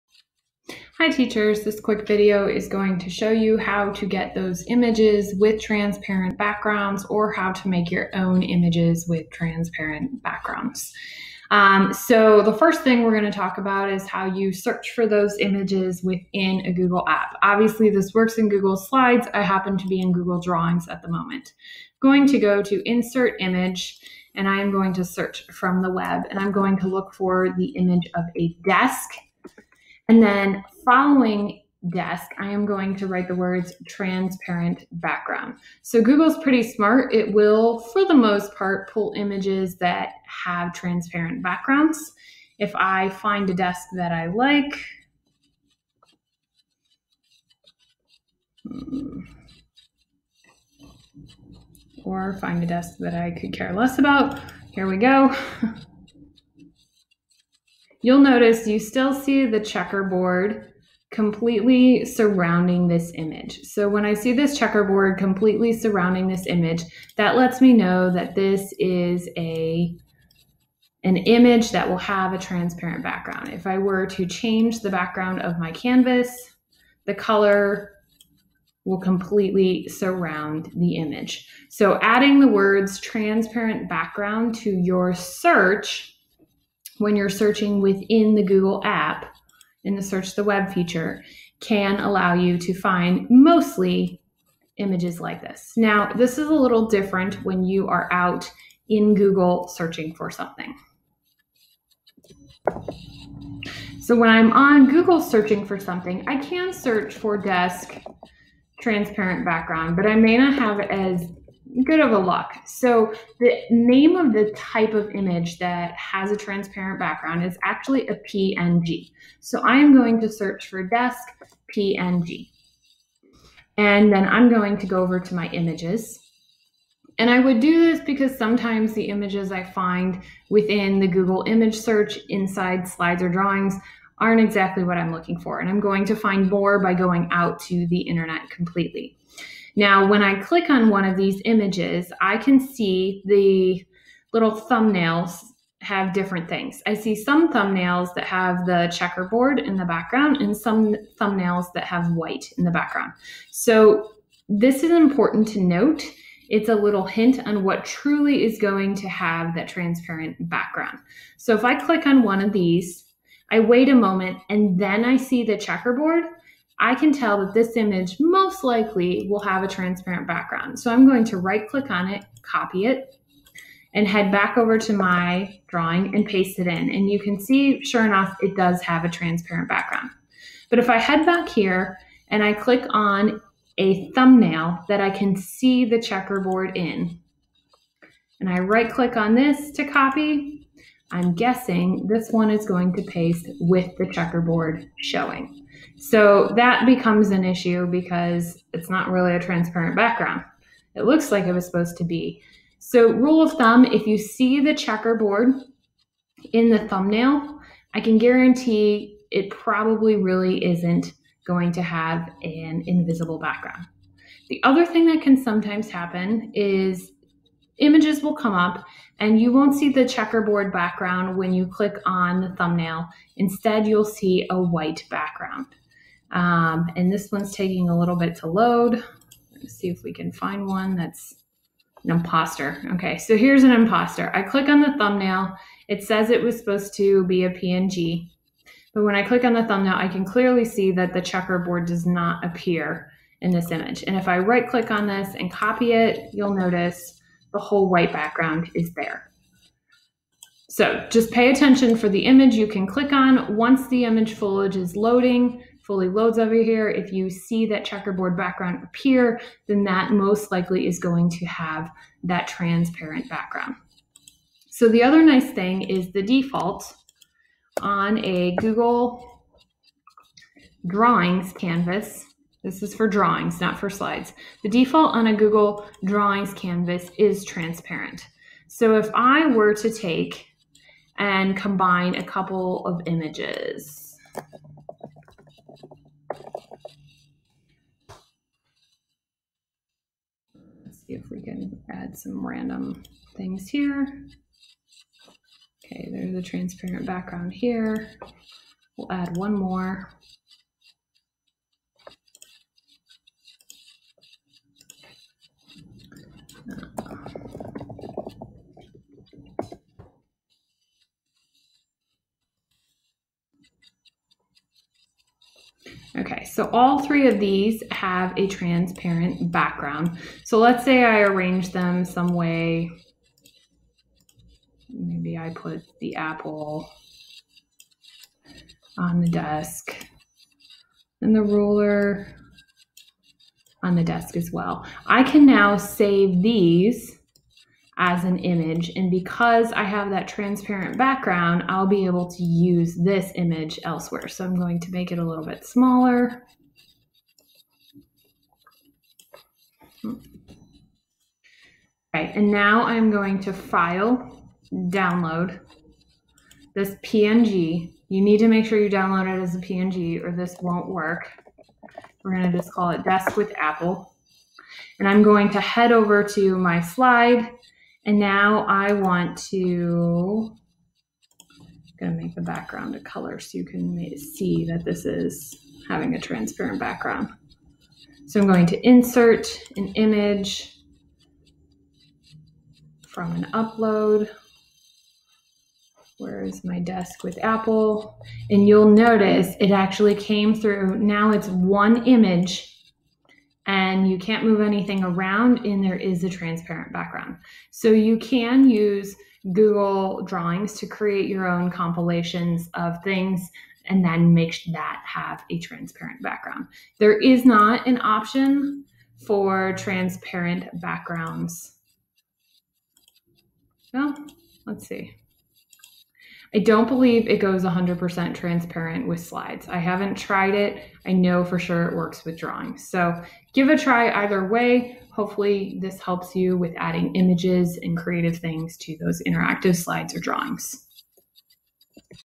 Images with Transparent Backgrounds
Learn how to pull images with transparent backgrounds by searching in Google Apps, on Google.com, or creating your own.